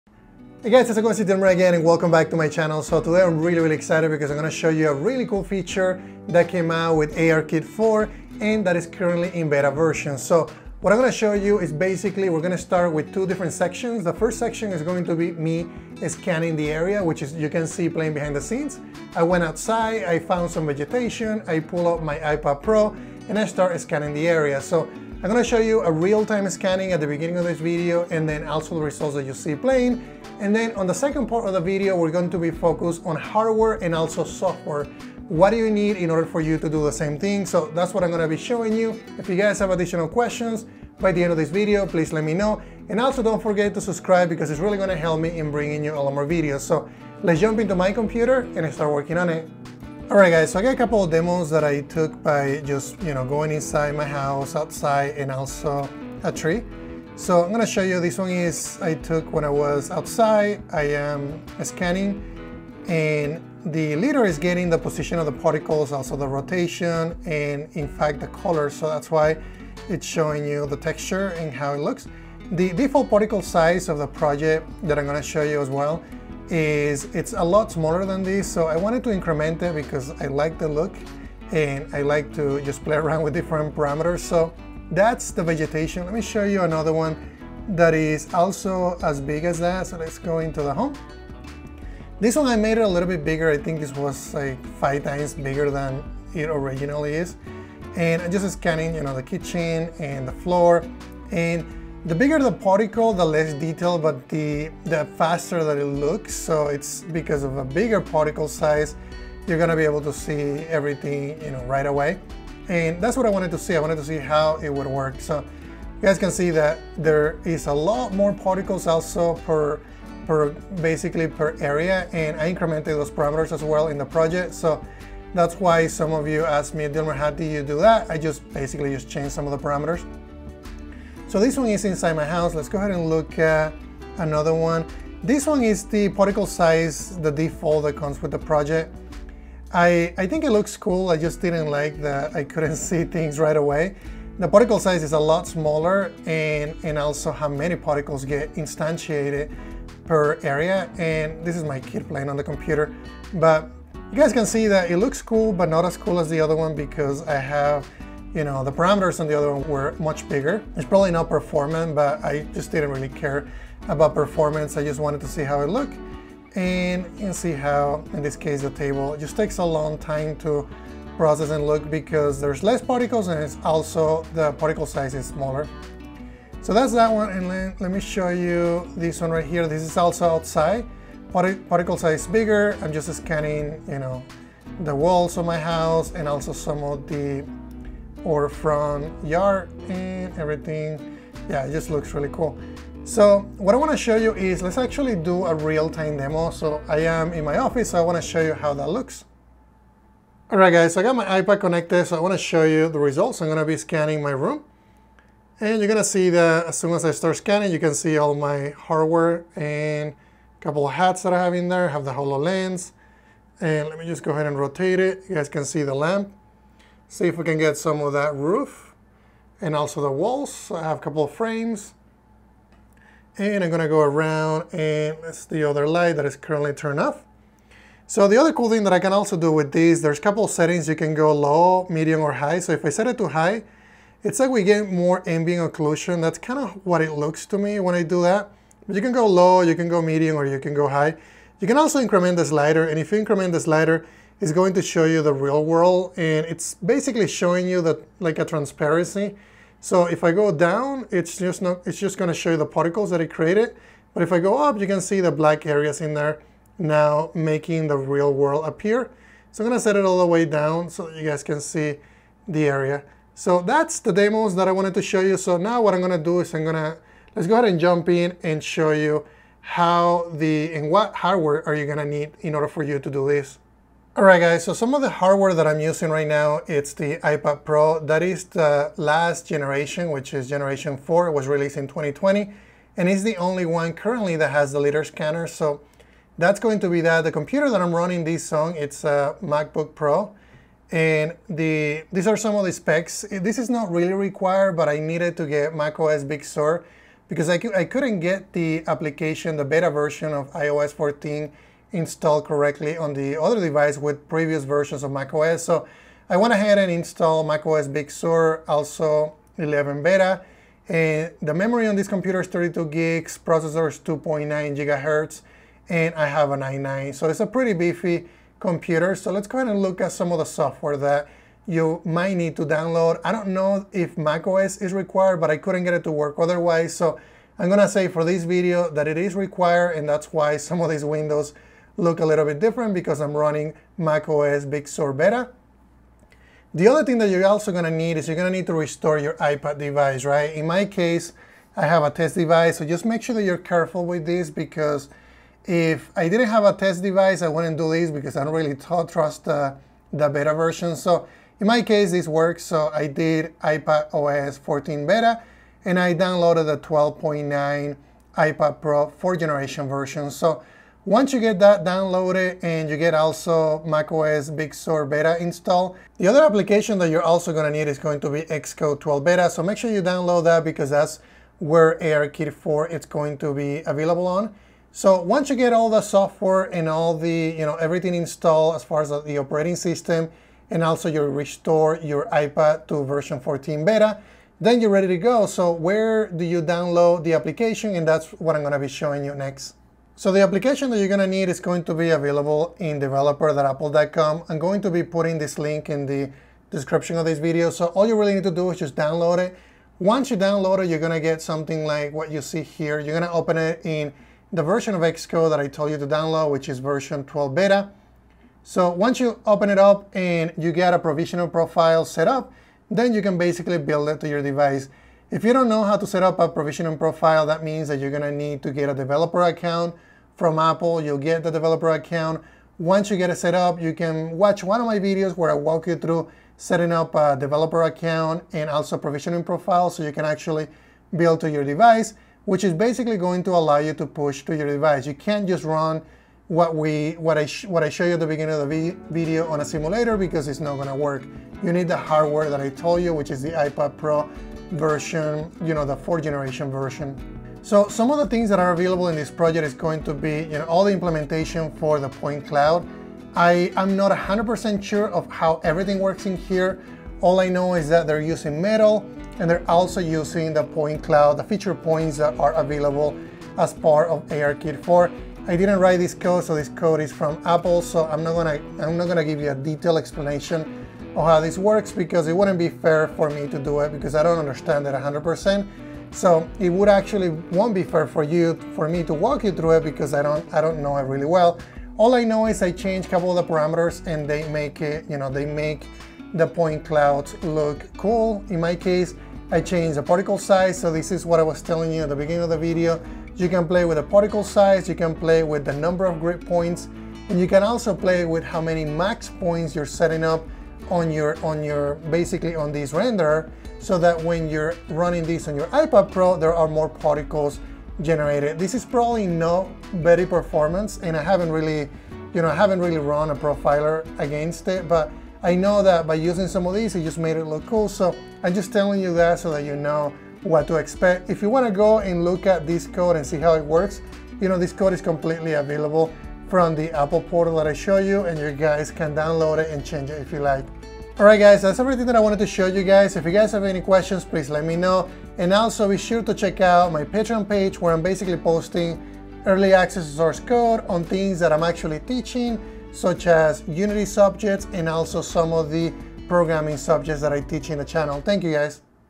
How To Generate Point Clouds With ARKit 4 iOS 14 Beta And iPAD Pro 4th Generation?
Today I show you an experiment I did by modifying few particle point parameters in Apple's point cloud example released during WWDC 2020.